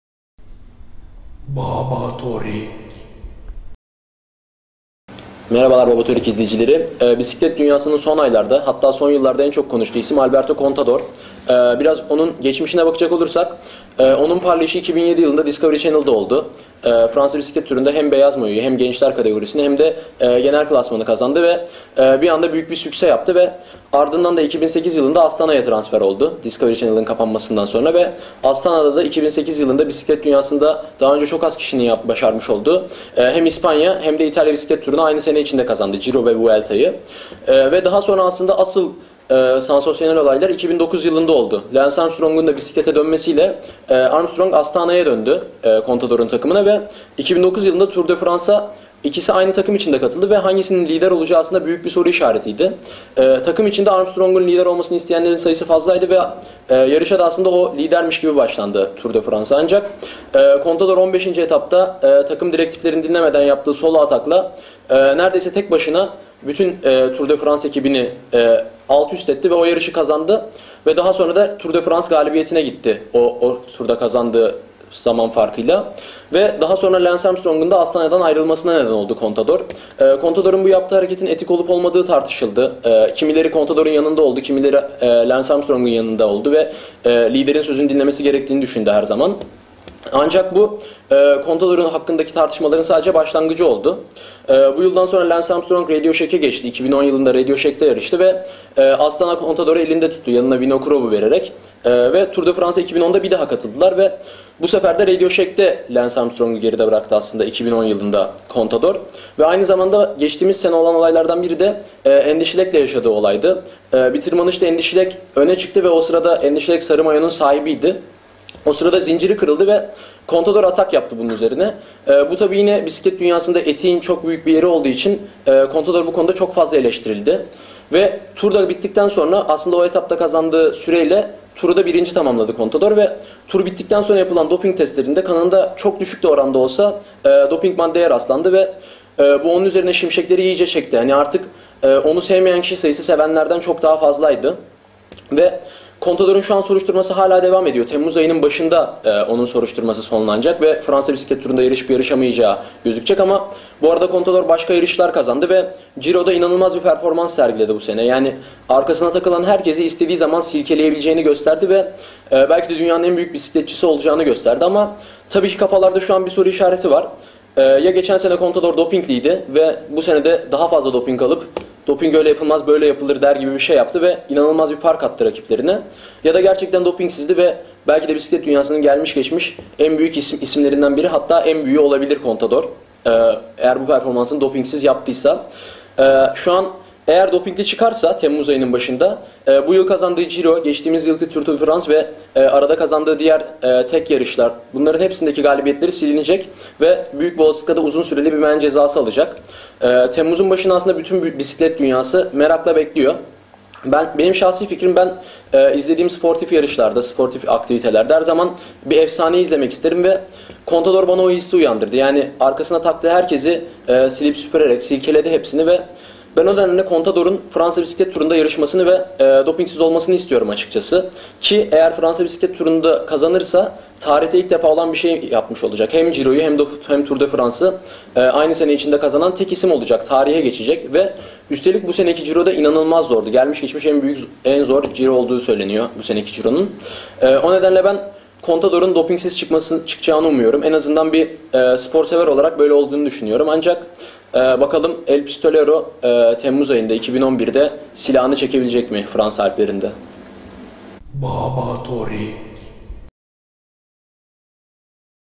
Babatorik Alberto Contador Degerlendirmesi 3 Haziran 2011
guney anlatiyor 3 Haziran 2011 Alberto Contador Degerlendirmesi
El Pistolero Fransa'da silahini cekebilecek mi?